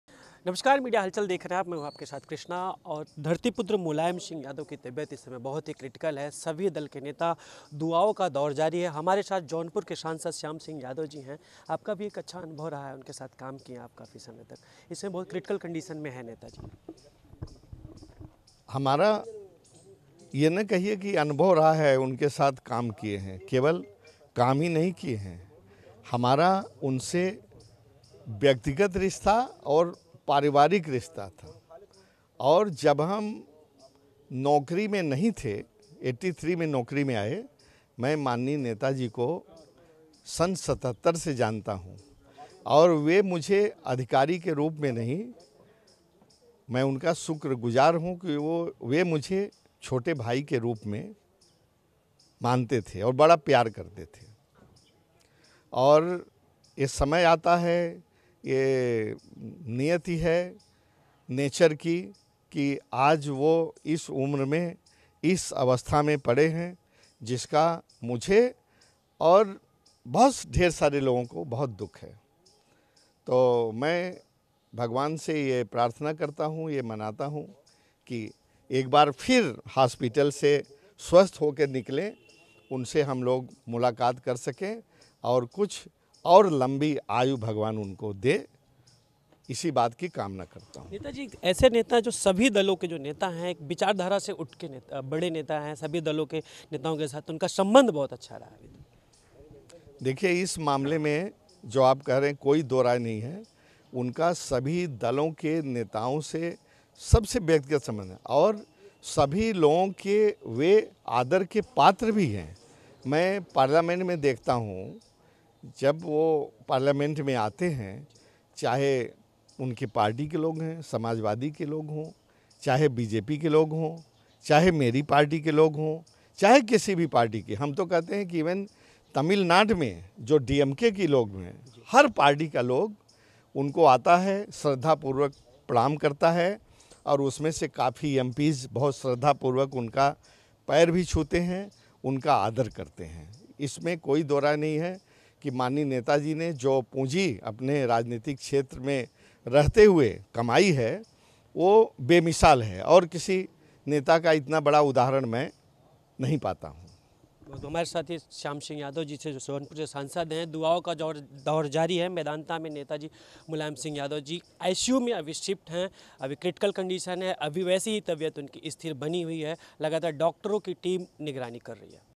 सांसद श्याम सिंह यादव नेताजी के साथ बिताए पलो को याद कर हुए भावुक और कहा नेताजी यौद्धा हैं !
जौनपुर से सांसद श्याम सिंह यादव नेताजी के साथ बिताए पलो को याद कर हुए भावुक और कहा नेताजी यौद्धा हैं !